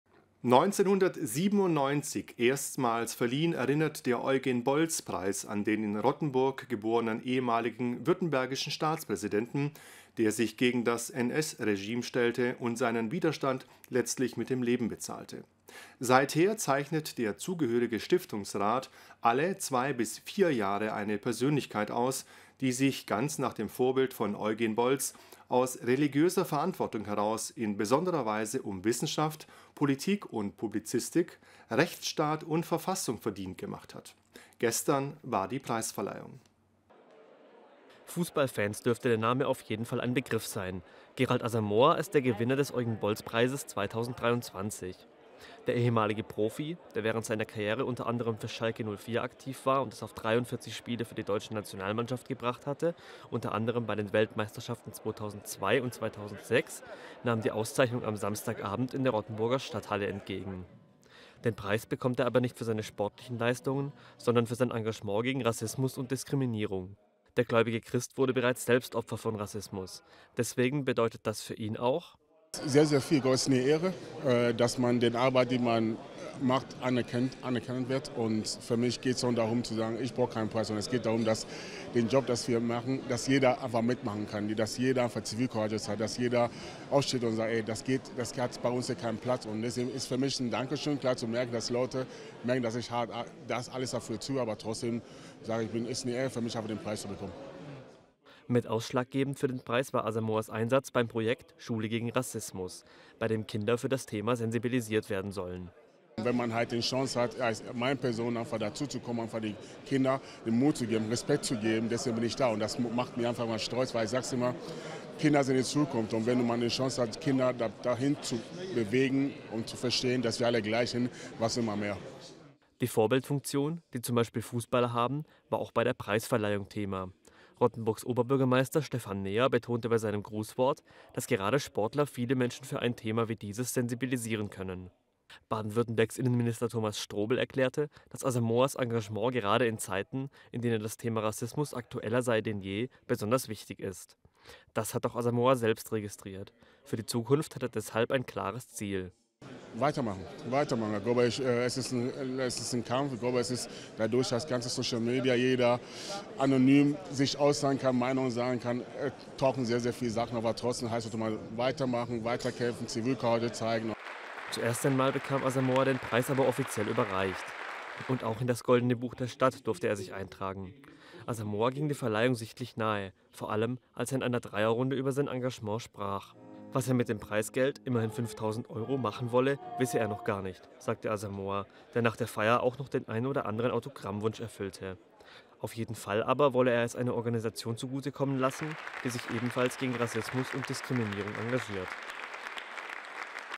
Ex-Nationalspieler Gerald Asamoah erhält Eugen-Bolz-Preis in Rottenburg
RTF.1-Nachrichten: Ex-Nationalspieler Gerald Asamoah erhält Eugen-Bolz-Preis in Rottenburg